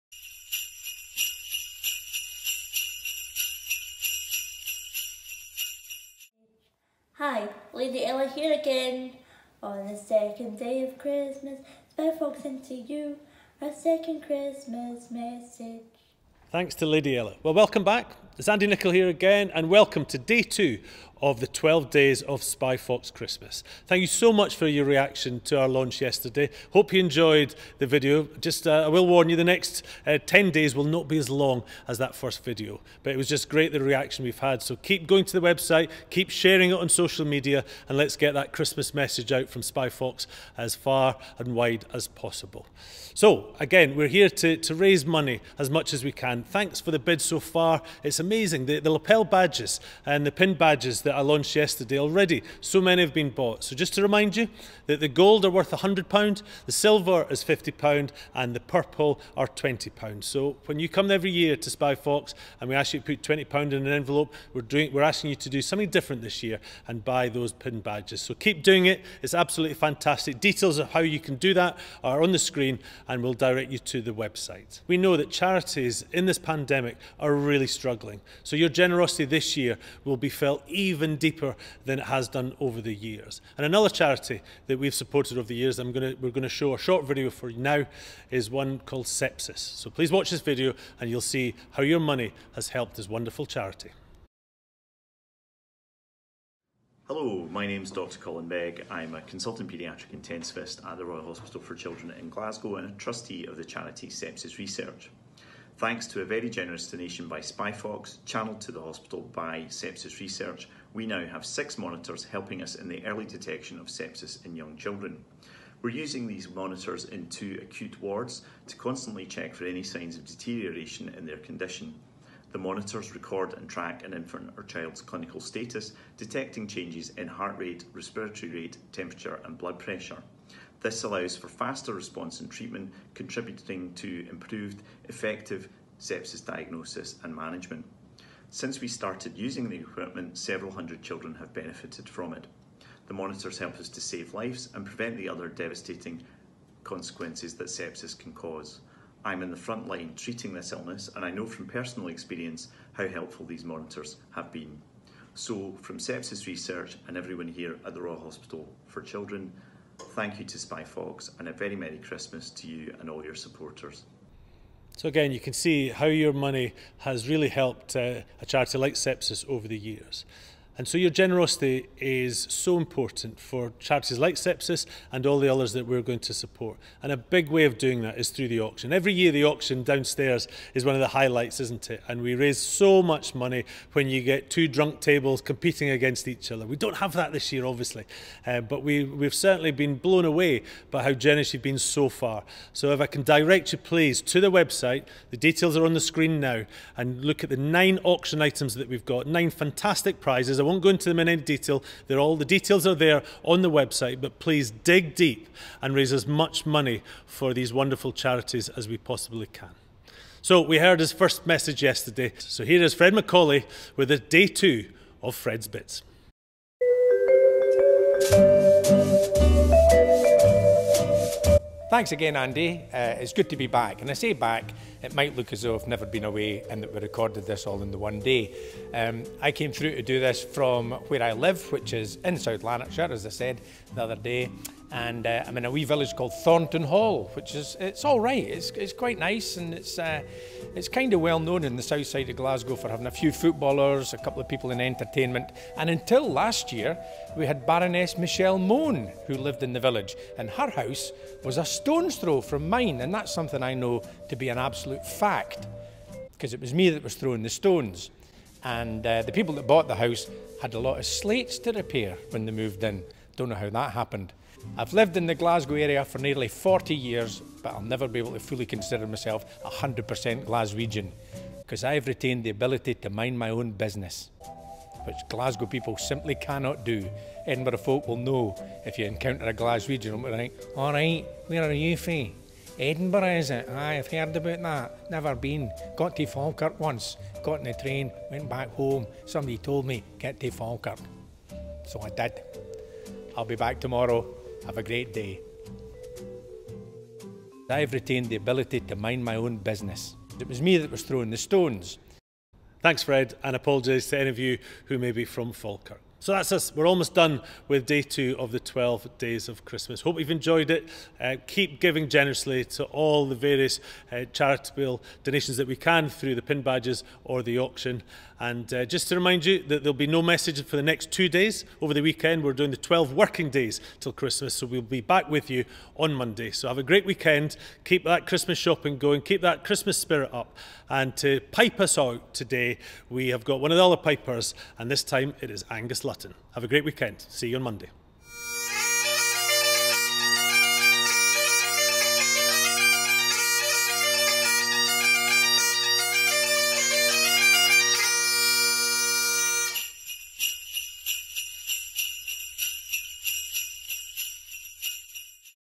Spifox Digital Christmas Day Two Appeal
The second day of our 12 day presentation hosted by Andy and Ella has Fred' Bit's back, features SEPSIS to enlighten is as to the need for your generosity, and an excellent close from Piper Angus Lutton